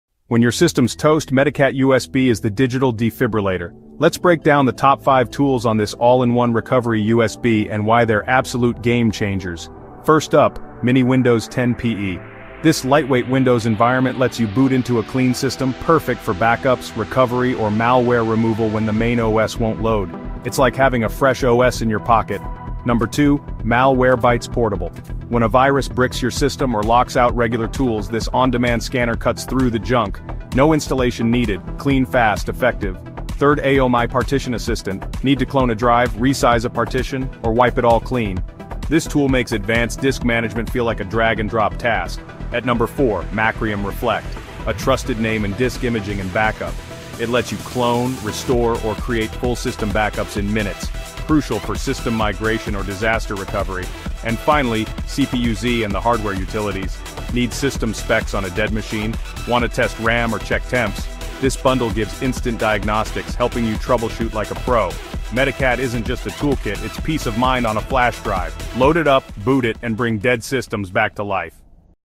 Top 5 reasons to use Medicat USB as your #1 Tool for PC repair and maintenance.
🛠️ Top 5 MediCat USB Tools You Need To Know | PC Recovery & Repair Essentials

If you're in tech support, PC repair, or just love having a rescue toolkit at the ready — this video is for you.

In this quick deep dive, we break down the top 5 most powerful tools inside MediCat USB, a must-have for troubleshooting, data recovery, malware removal, and more.

💡 Featured Tools:
1️⃣ Mini Windows 10 PE
2️⃣ Malwarebytes Portable
3️⃣ AOMEI Partition Assistant
4️⃣ Macrium Reflect
5️⃣ CPU-Z + Hardware Utilities

Whether you're fixing a friend’s laptop or rescuing your own system from disaster, MediCat USB is your secret weapon.

🔧 Boot it. Fix it. Save the day.